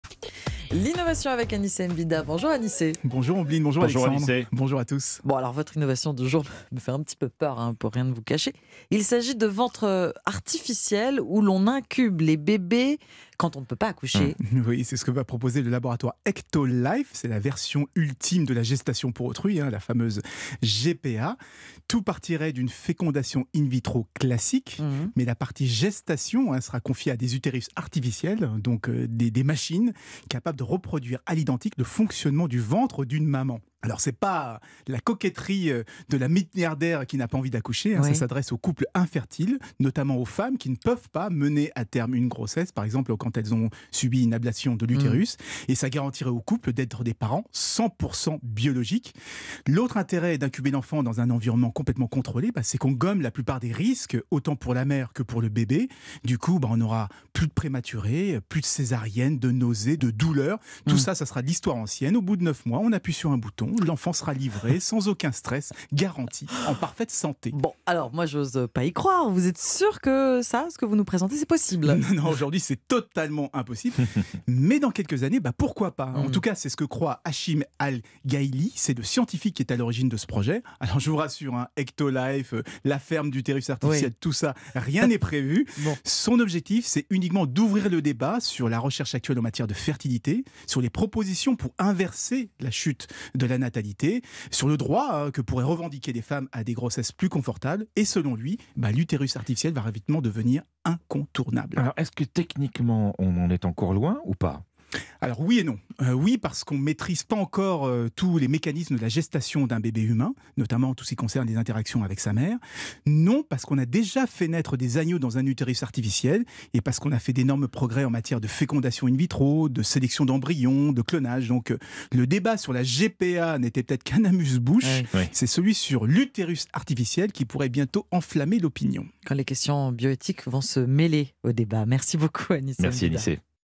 Des ventres artificiels où l’on incube les bébés quand on ne peut pas accoucher
Anicet Mbida nous livre chaque matin ce qui se fait de mieux en matière d'innovation.